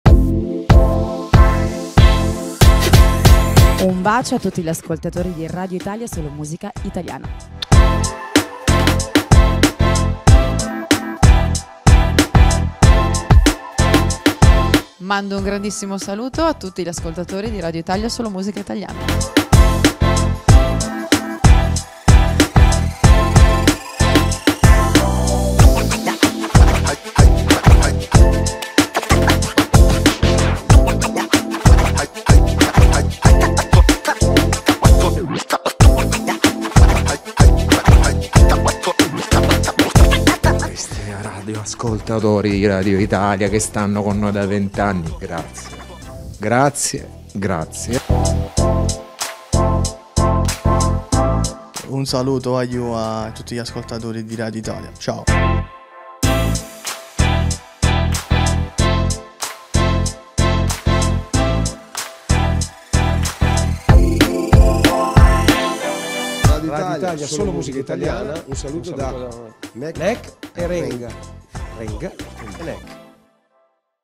Best of Fuori Sanremo ING - Giorno 2
Seconda giornata di interviste al Fuori Sanremo ING. Guarda il video best

Benvenuto nel canale ufficiale di Radio Italia!

Seguici anche su tutti i nostri canali social